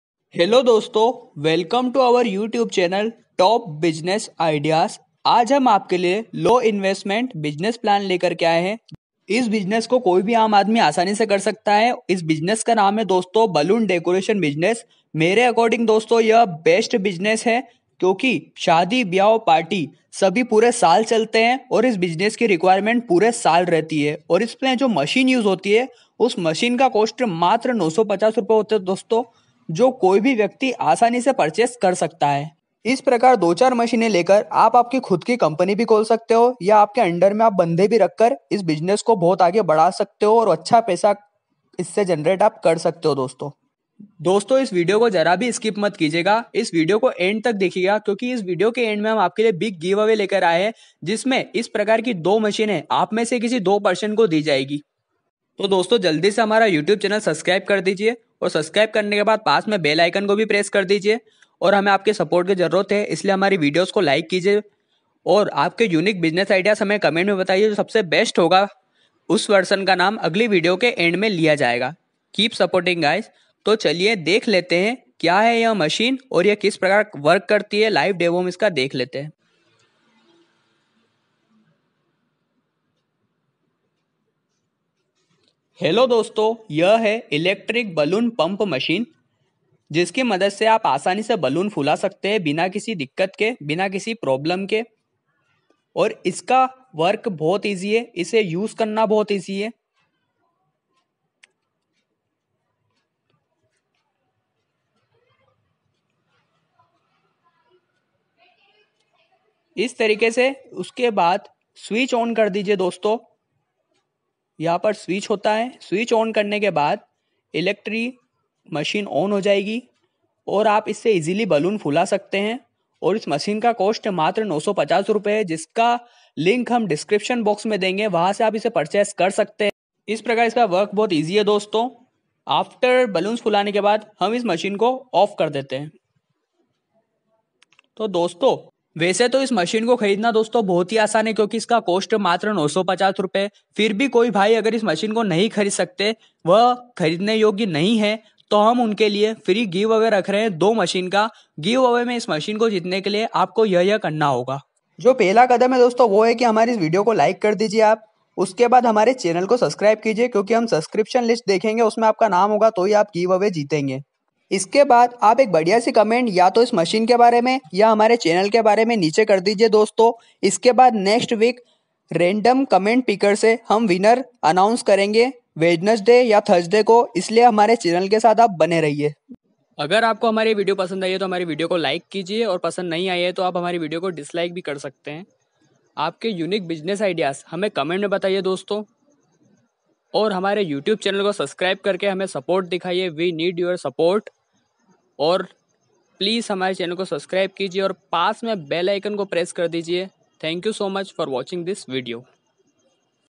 सिर्फ 950₹ की मशीन | रोज का 2000 fix | Small business idea | balloon decoration | TBI
हम आपके लिए एक खास business idea लेके आए हे जिसे आप  bht आसानी से करके self depend हो सकते हे।।।।
और व्यापार को आगे बड़ा सकते हे

Small business idea 2019
सिर्फ 950₹ की मशीन और रोज का
2000₹ आराम से आप कमा सकते कही भी 1 2 घण्टे decoration करके।।।।
मशीन यहाँ से ख़रीदे➡➡➡➡
उम्मीद है आपको हमारा idea पसंद आया होगा

Like करे वीडियो को आपसे निवेदन है और चेंनल को आगे बढ़ाए

किसी प्रकार के प्रश्न के लिए हमे mail करे।।।